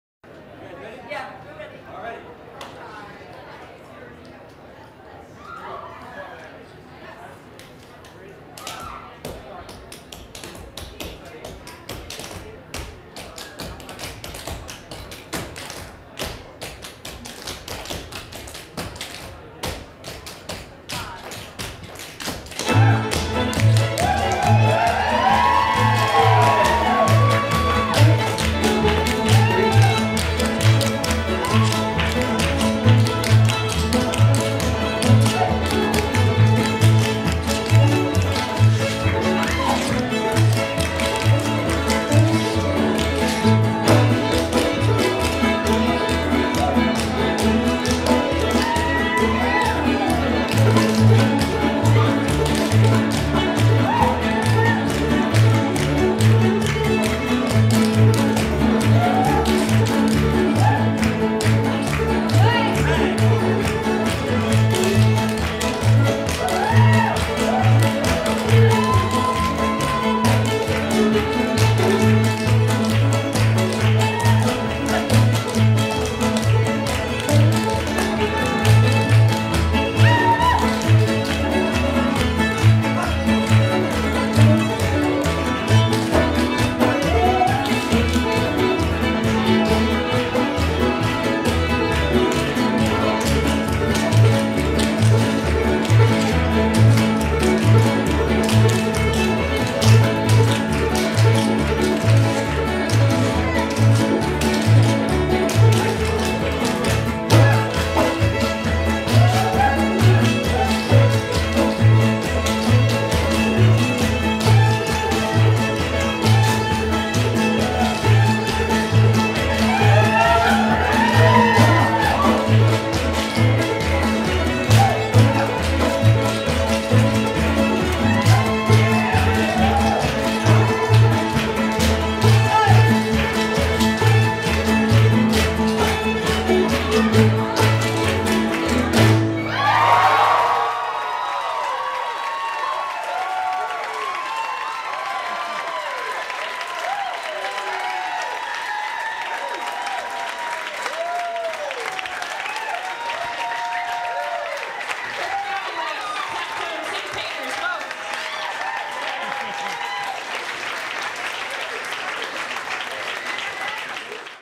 TapTone Syncopators at the Tractor
The 25th Reunion Tour of the TapTone Syncopators. The TapTone Syncopator perform at the Tractor Tavern in Seattle on July 14, 2008. The TapTones were the first precision clogging group in the Pacific Northwest, and were active in the early 80's.